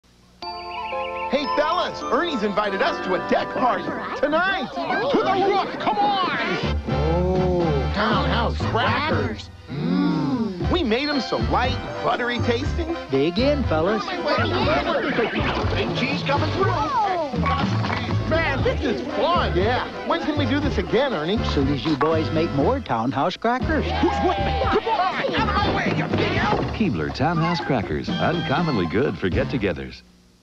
Town House ad, 2000
The Keebler elves pitch their cracker brand that's "uncommonly good for get-togethers".